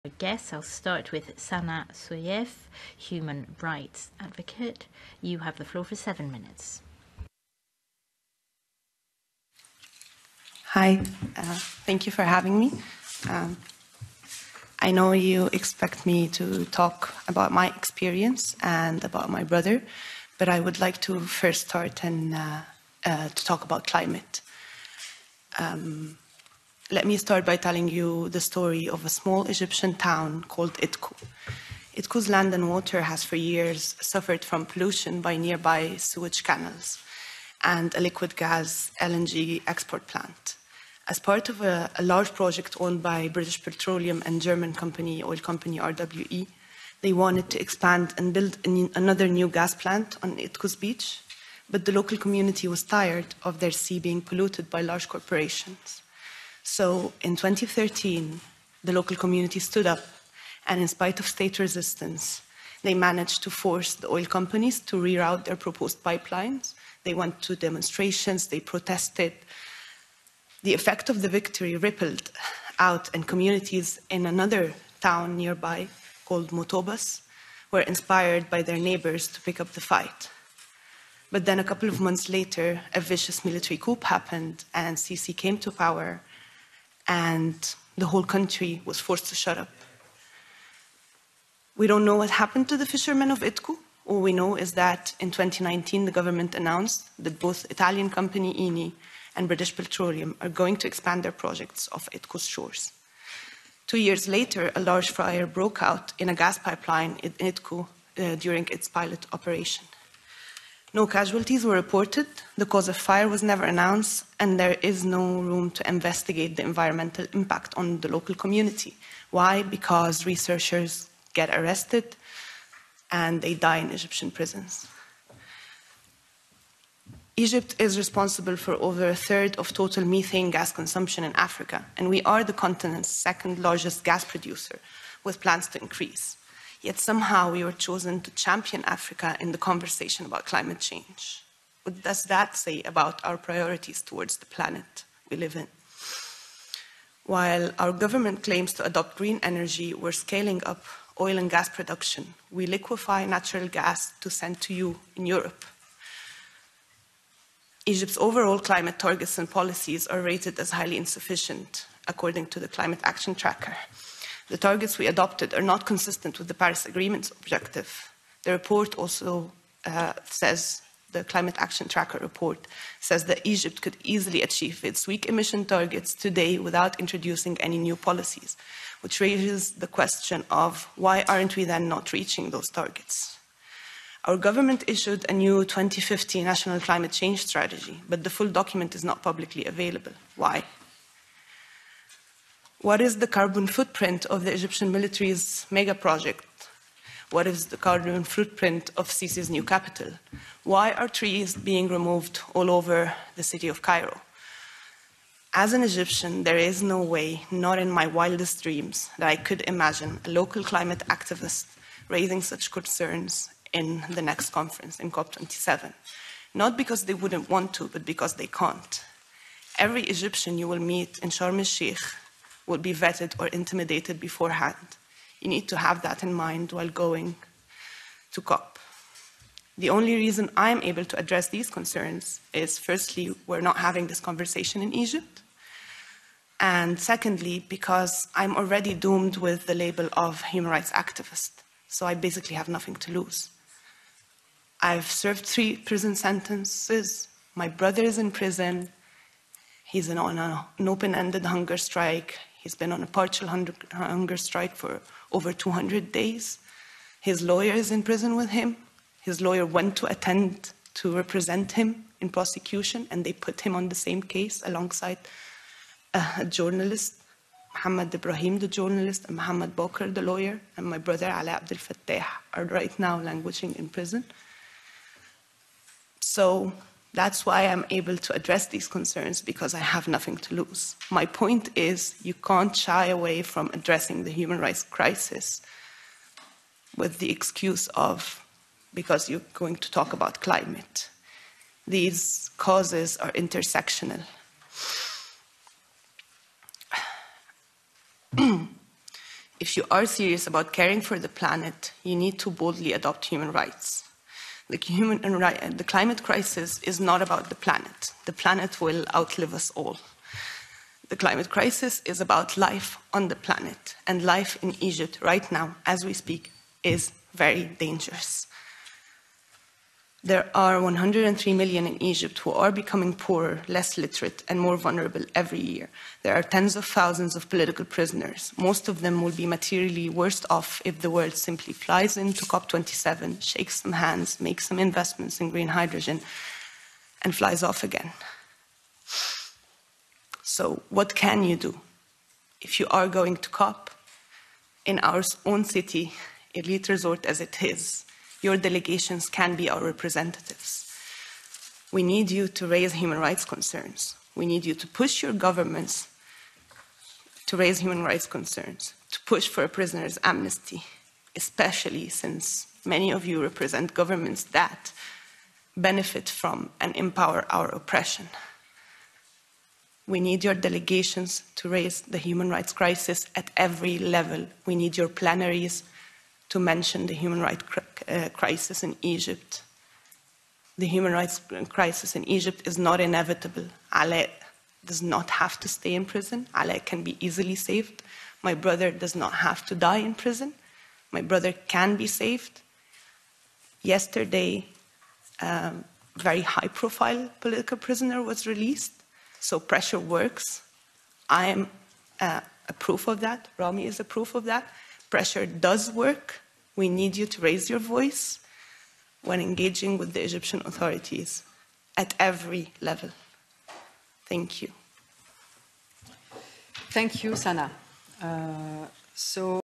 Sanaa Seif addressing EP Subcommittee on Human Rights
On October 26th 2022 Sanaa Seif, Alaa Abdel Fattah's younger sister and a former political prisoner herself, was invited to address the European Parliament's subcommittee on Human Rights, to talk about the Human Rights situation in Egypt prior to Cop27